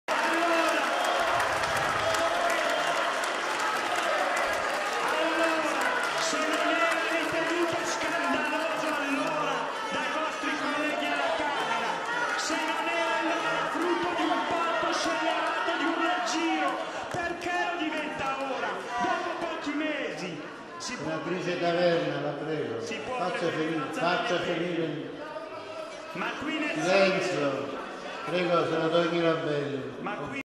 VOTO DI SCAMBIO FINOCCHIARO URLA BUGIARDI A SENATORI M5S IN AULA
Il Senato ha approvato in via definitiva il ddl sul voto di scambio in una seduta molto movimentata e chiusa in un clima di bagarre. Già durante le dichiarazioni di voto i parlamentari del gruppo Cinquestelle avevano più volte interrotto i colleghi, esponendo foto di Napolitano e Berlusconi e gridando 'collusi', tanto che il presidente Grasso aveva sospeso la seduta ed espulso i M5S Santangelo e Airola. Dopo il voto, nuovi cartelli dei Cinquestelle con un fotomontaggio di Renzi e Berlusconi con la coppola e la scritta 'I padrini del voto di scambio'. Tensione anche alla Camera durante l'informativa di Alfano sull'immigrazione clandestina, espulso il leghista Prataviera.